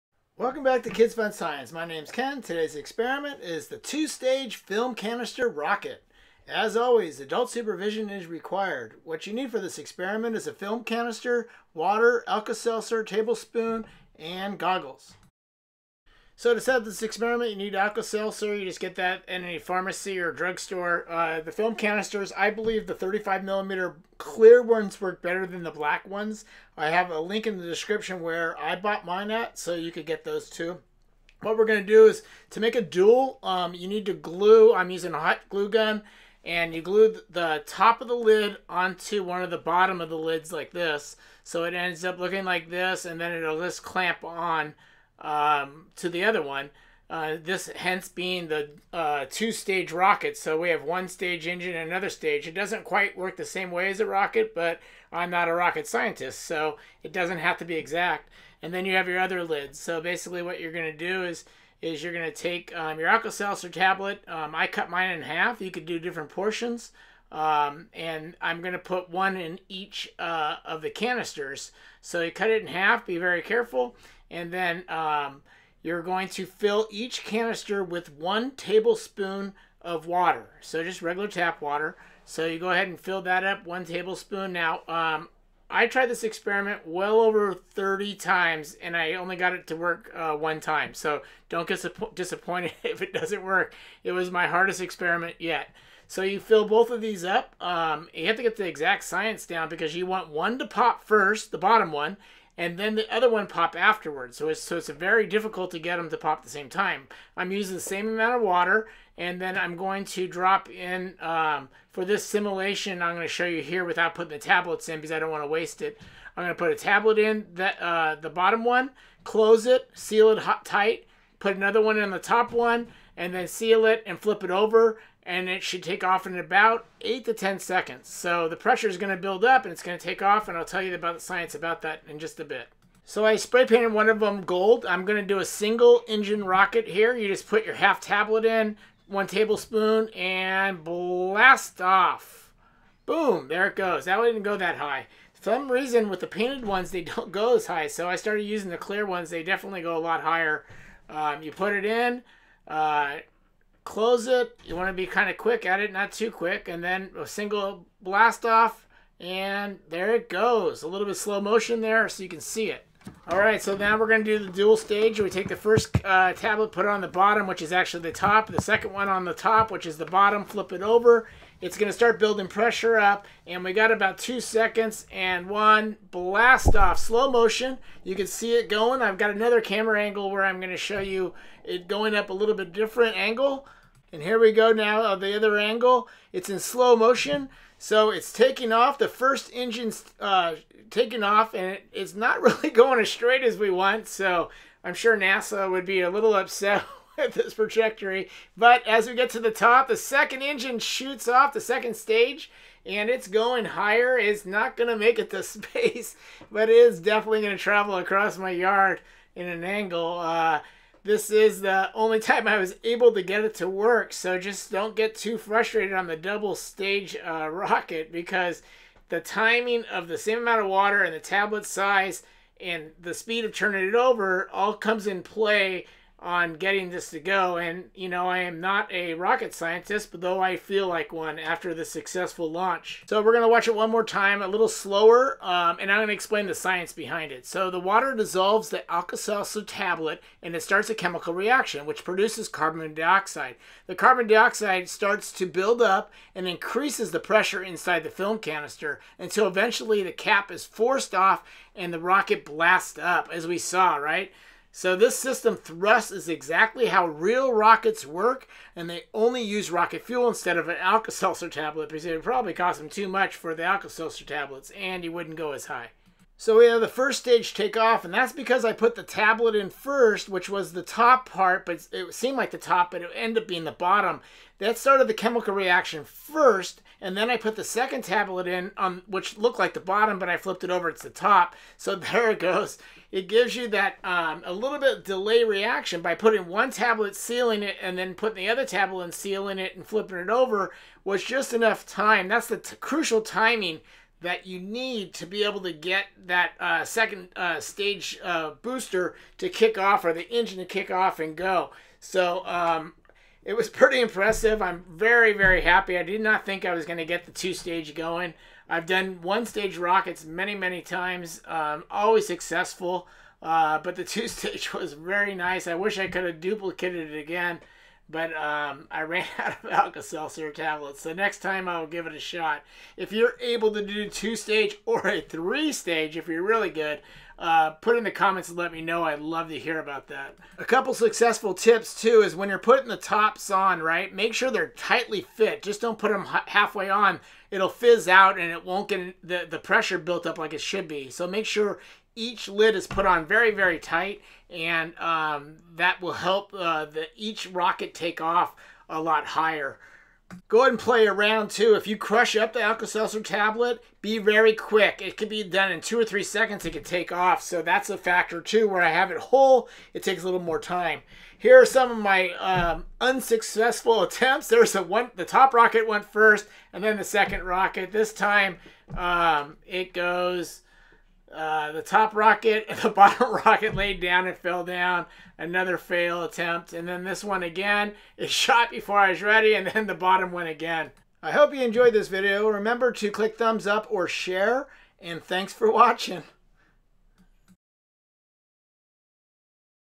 Two Stage Film Canister Rocket experiment
Film canister

Alka-Seltzer 24 count

You can do one stage Rocket or a Two Stage Rocket. It took me over 30 tries to get it to get the Two Stage Rocket to work. This was the hardest experiment I have ever done!. I still don't think NASA will hire me even after my great success. : )

Caution: if it does not launch, wait at least thirty seconds before examining the canister. Usually the cap is not on tight enough and the carbon dioxide has leaked out instead of building up.

The water dissolves the Alka-Seltzer tablet and starts a chemical reaction, which produces carbon dioxide. The carbon dioxide starts to build up, which increases the pressure inside the film canister, until eventually the cap is forced off and the rocket blasts up. This system of thrust is exactly how real rockets work, only they use rocket fuel instead of Alka-Seltzer.

Tips
Make sure the film canister lid is tightly fitting or you will only get a disappointing 'fizz'. You should also clean the canister lip and lid between demonstrations so that no pieces of Alka-Seltzer get stuck between them, ruining the seal.

Serving Suggestions
I used a 1/2 tablet but try a whole one, or even crush the tablet up. The crush tablet will speed up the chemical reaction and it will pop quicker so make sure your pretty fast putting on the lid. : )

Where to buy Alka-Seltzer

Where to buy White Plastic Film Canisters